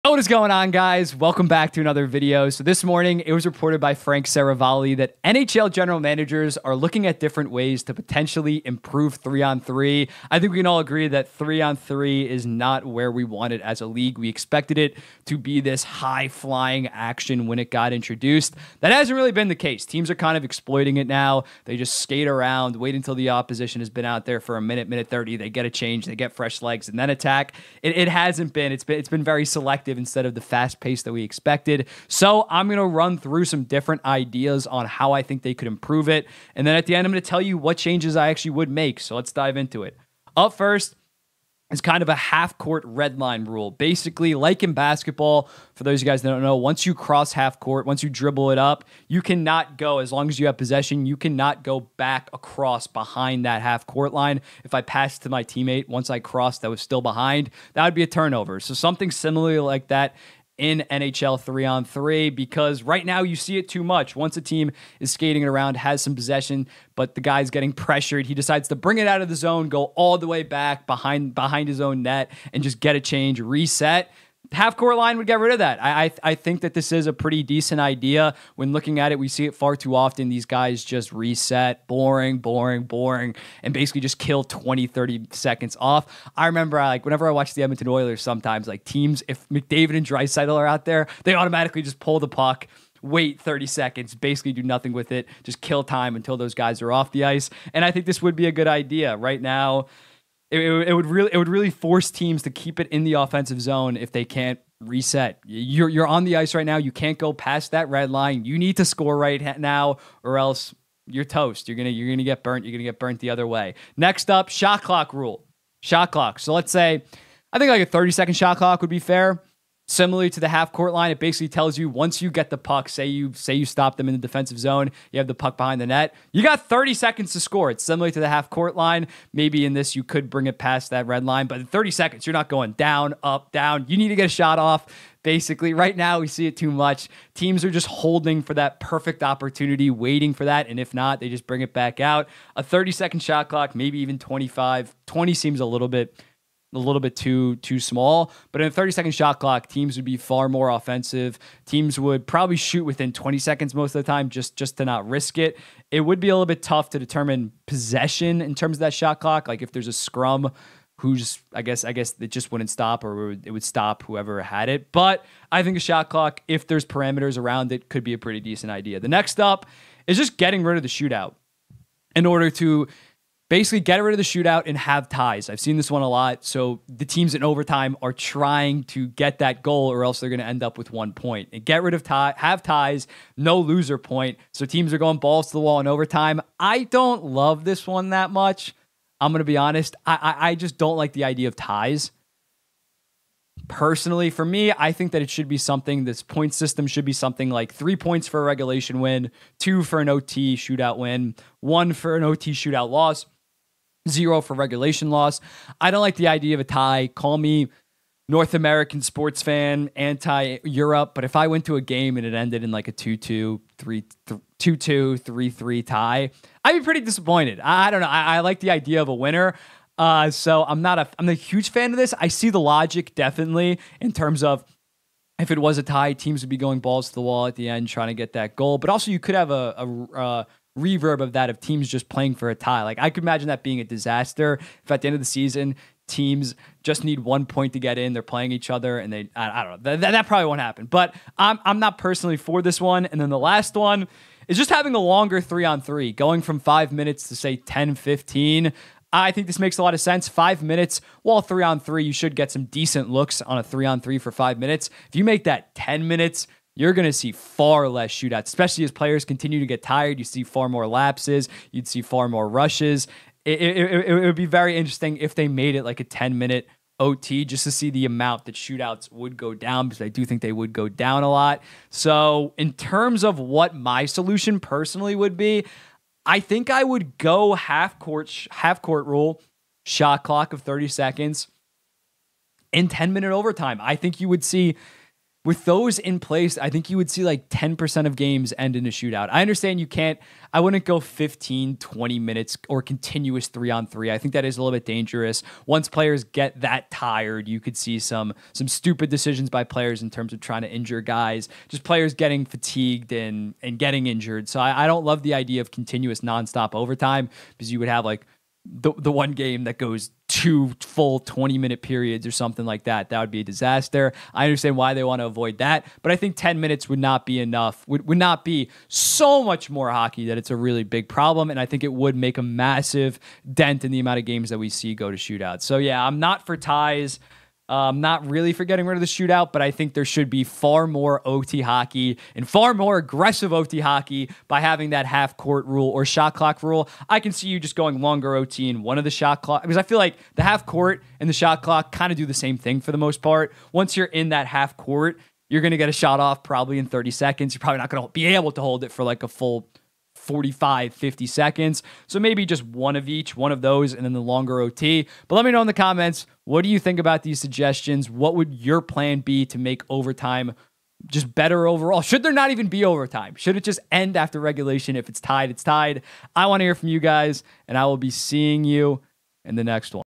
My Plan To FIX The NHL's 3 on 3 OT
According to Frank Seravalli the NHL's GM's are discussing to improve 3 on 3 OT, I run thru some solutions and at the end give my full plan.